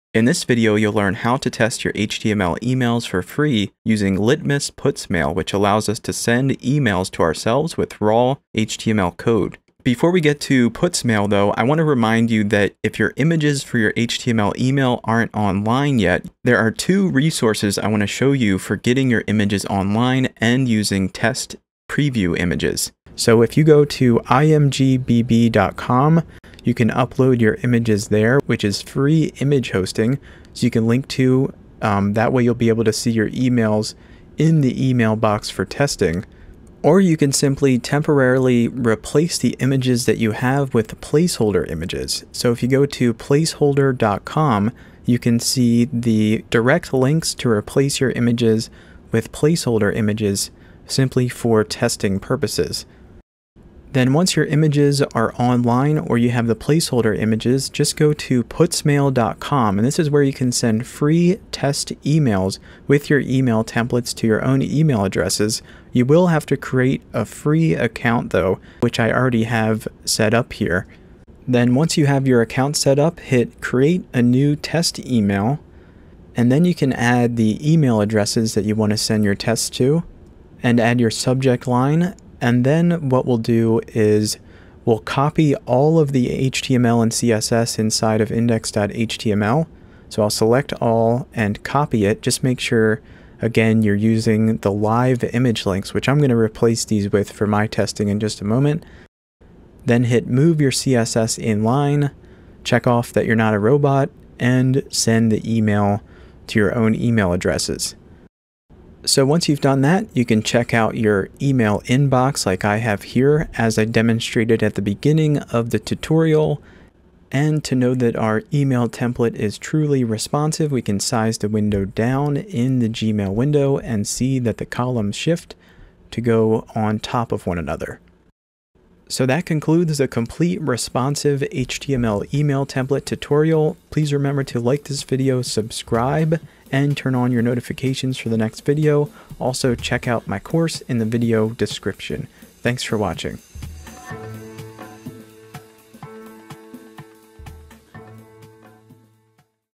Litmus Putsmail for HTML Email Preview Testing
New & Updated for 2024 with the latest HTML Email concepts.

Litmus Putsmail is a great tool for testing your HTML Emails for free if you don't have access to the full services of Litmus Email Client Preview Testing or their competitor Email On Acid.